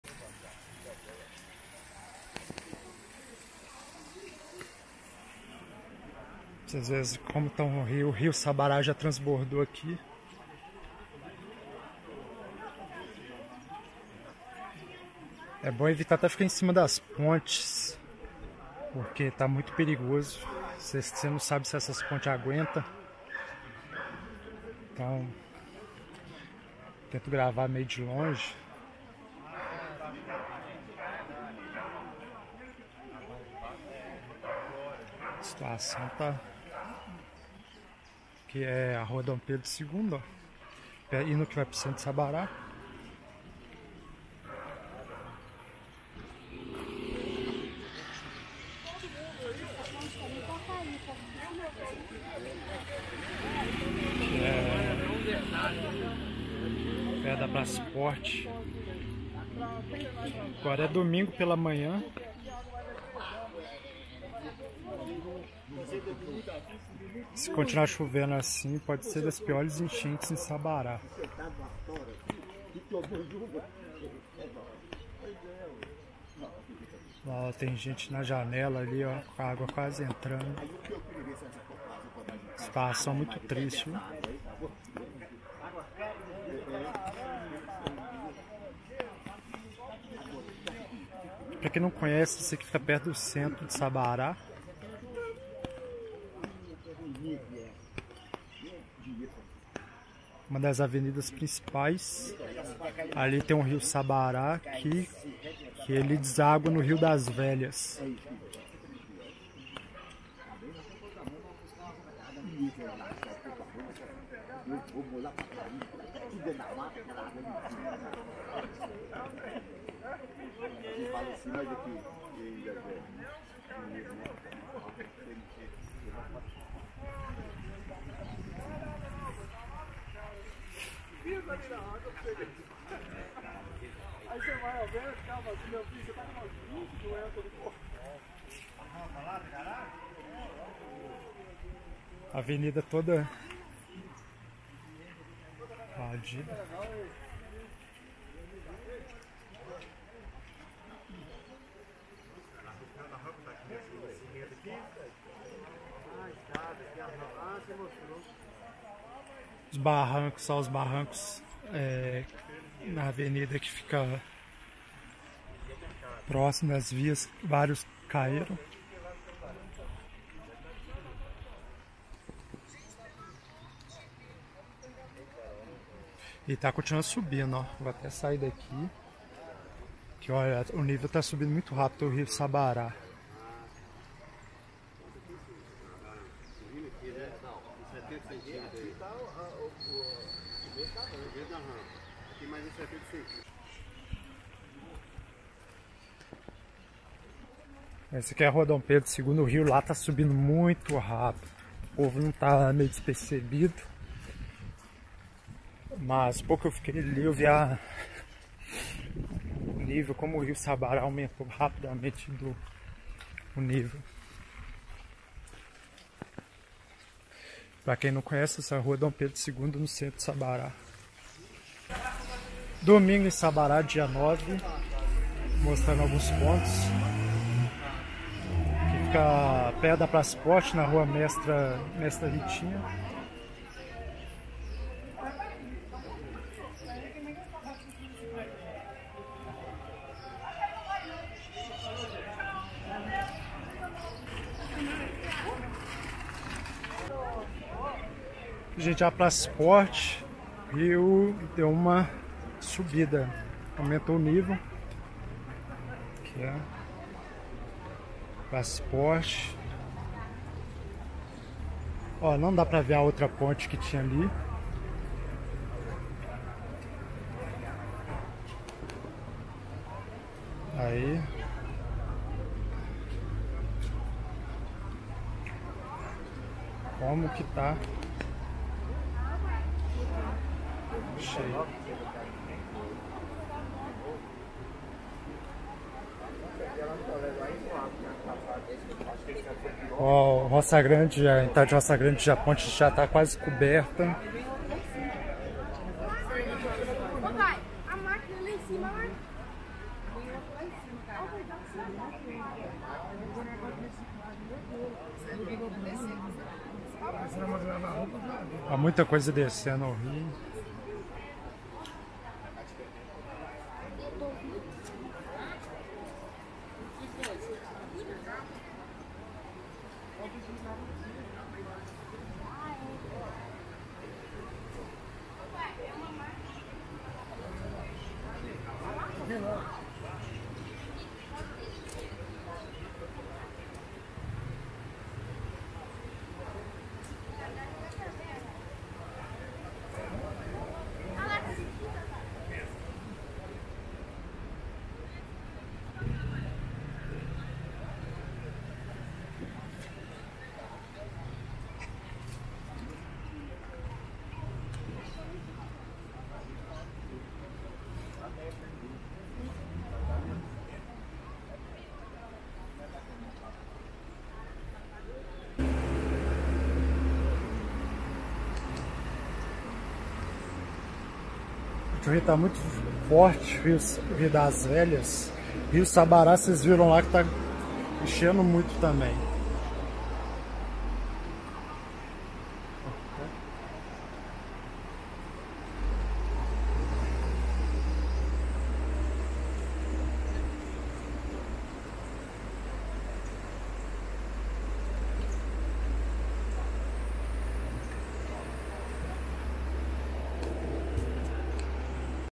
Enchente em Sabará - 9 de janeiro de 2022 - 11h
Triste situação de nossa cidade. Sabará-MG

Informações da Enchente em Sabará, Minas Gerais